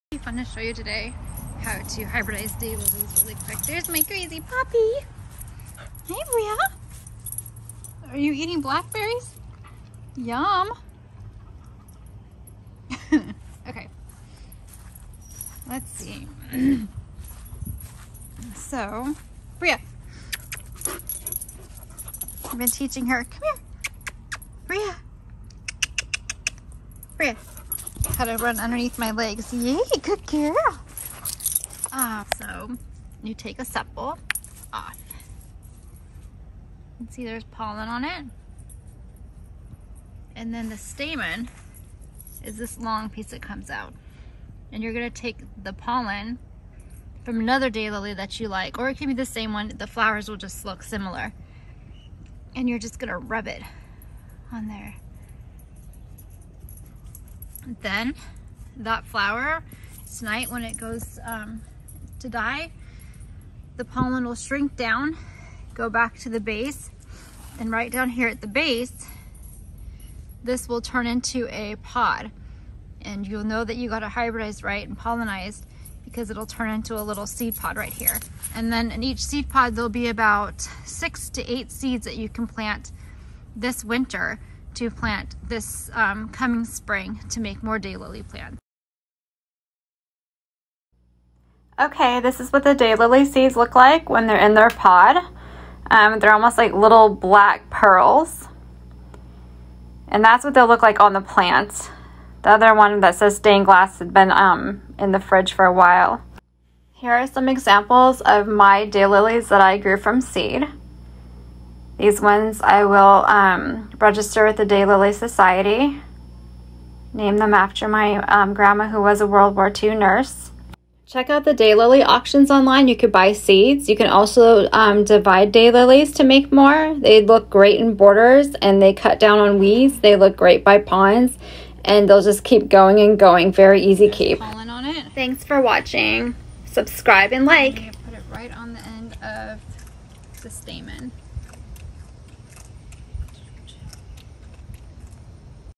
How to grow Daylily Flowers from seeds
Quick tutorial of how to hybridize daylily flowers to make a seed pod, then save seed to plant to make a garden full of day lilies.

For the complete instructions and walk through of growing daylily from seeds. :)

Bria my smooth Collie shows off a bit with a new trick, snacks on blackberries.